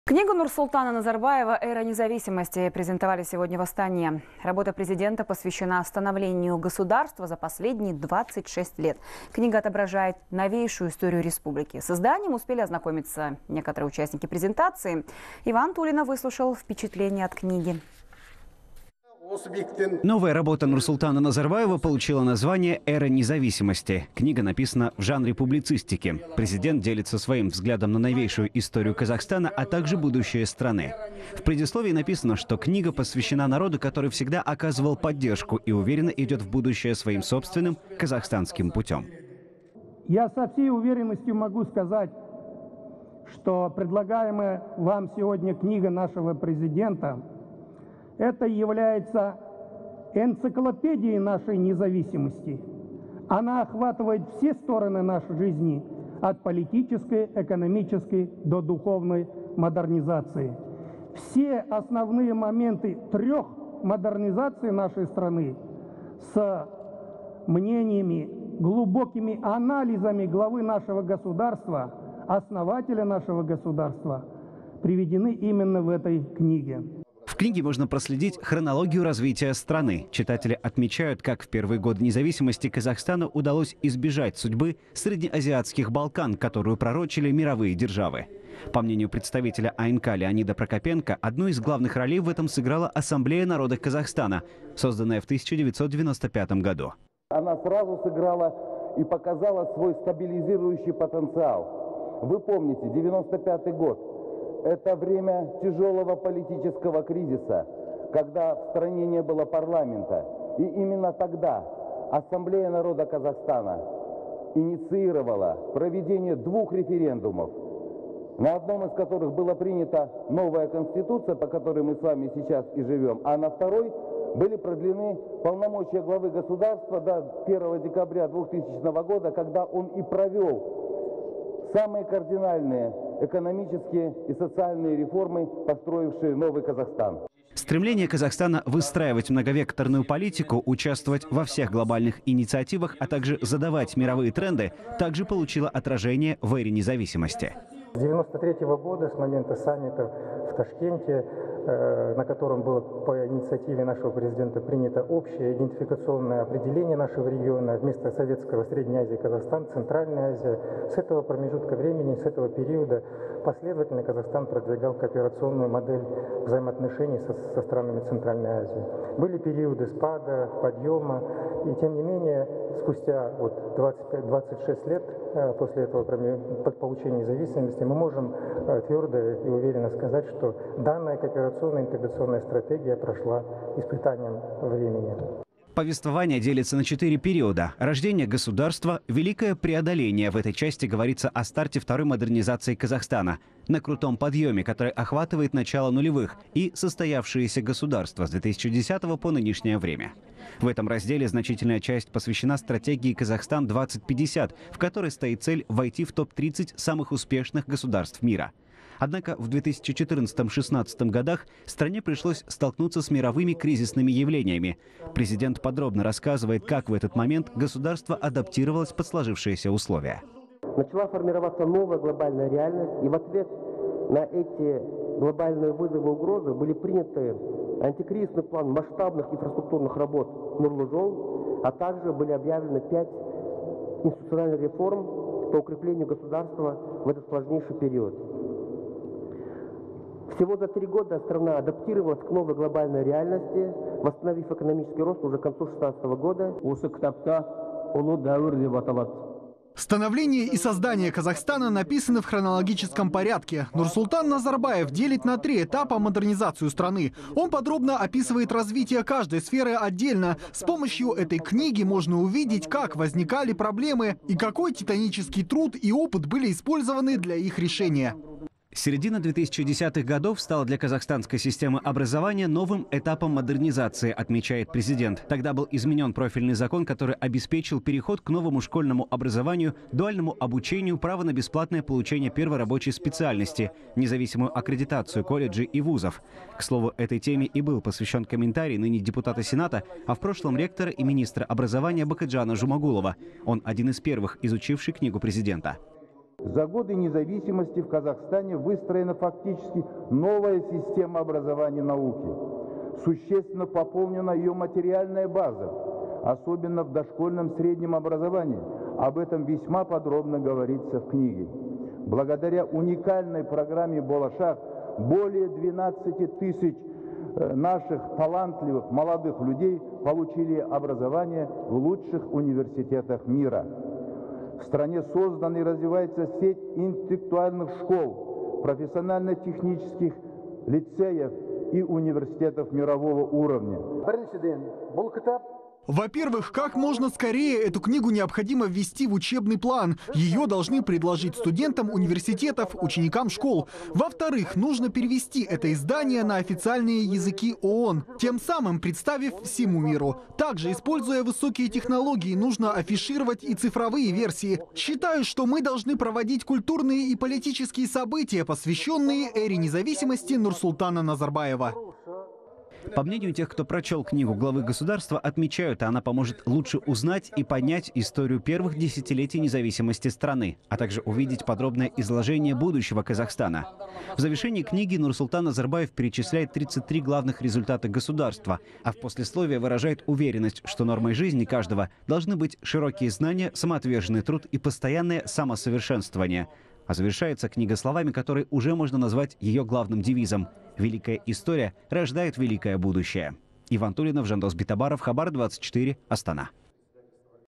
Книгу Нурсултана Назарбаева «Эра независимости» презентовали в Астане
Книгу Нурсултана Назарбаева «Эра независимости» презентовали сегодня в Астане.